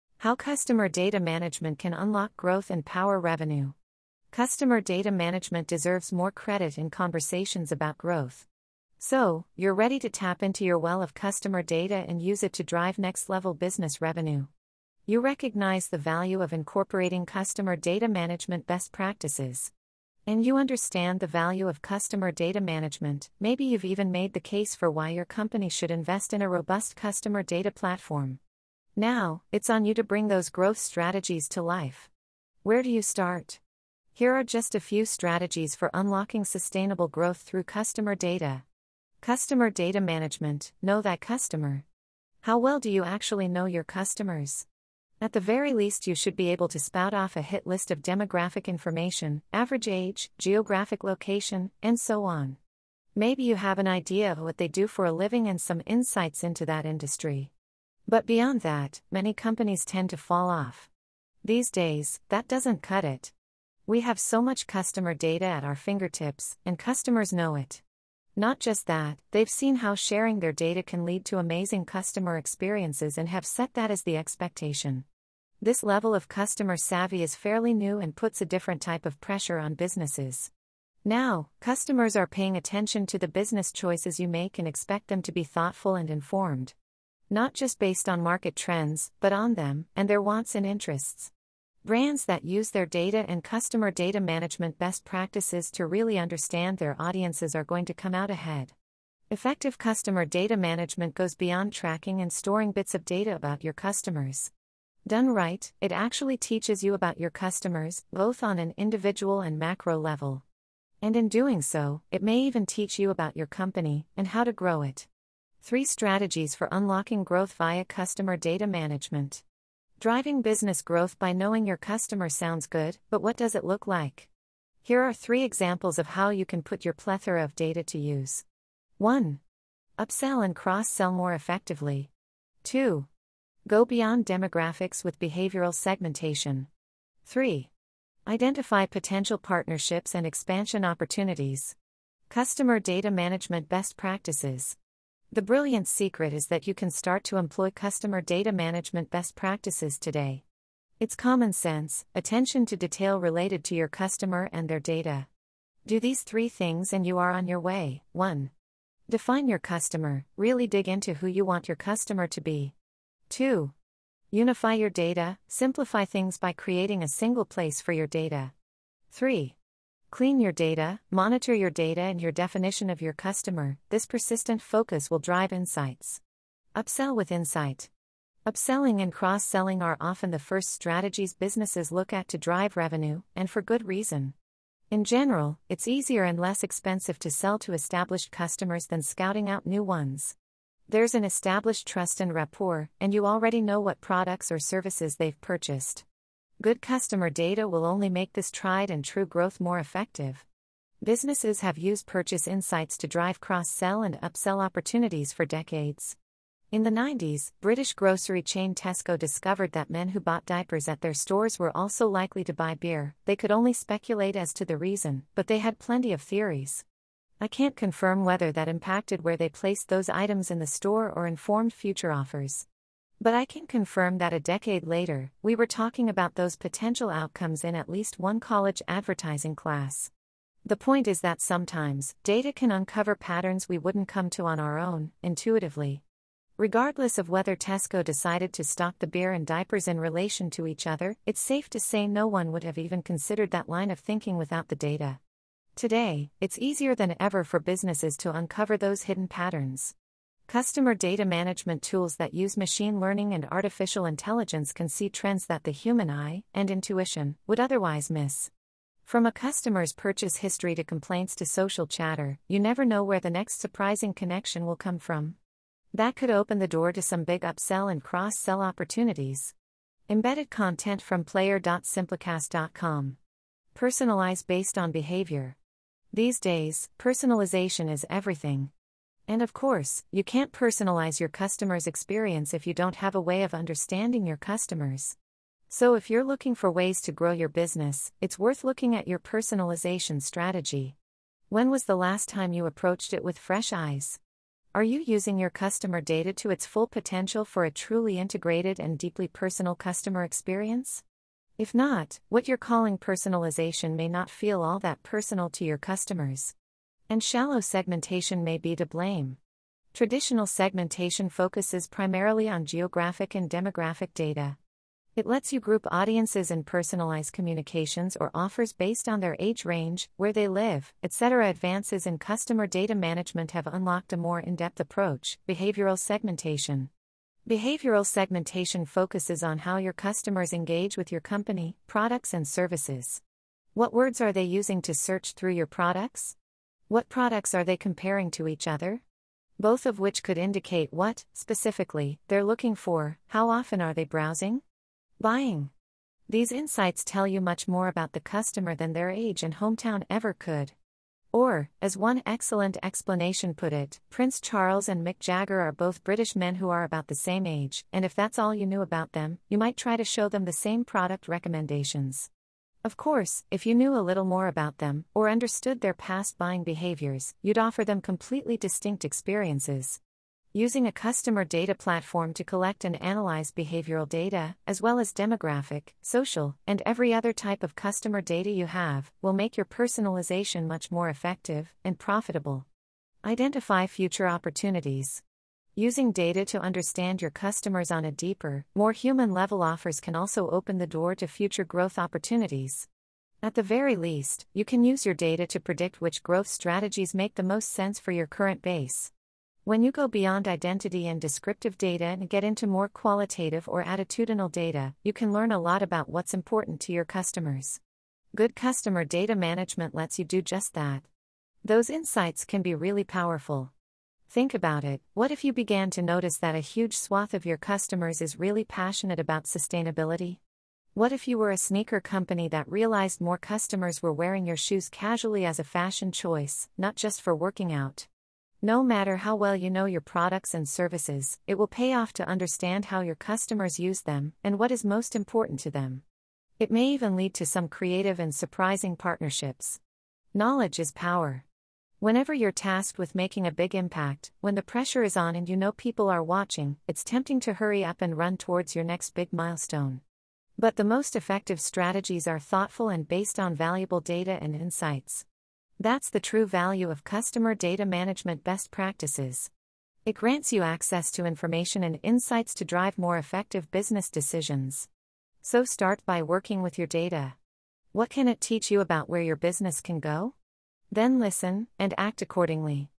How customer data management can unlock growth and power revenue
When you go beyond identity and descriptive data, and get into more qualitative or attitudinal data, you can learn a lot about what's important to your customers. Good customer data management lets you do just that.